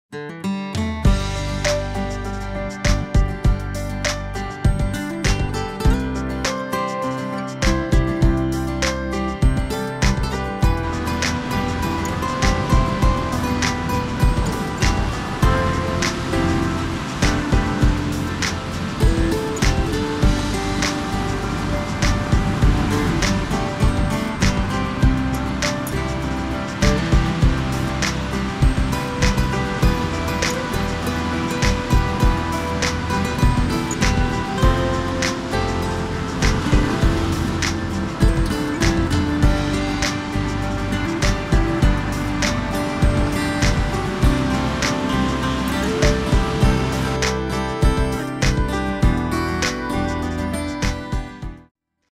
Good Morning At Beach
Yamandhu Marang ?
 (are you well)

Come sit with me for about 60 seconds and connect to country.

Hope you have a marang yiradhu ? (good day )

Video, Mark Saddler.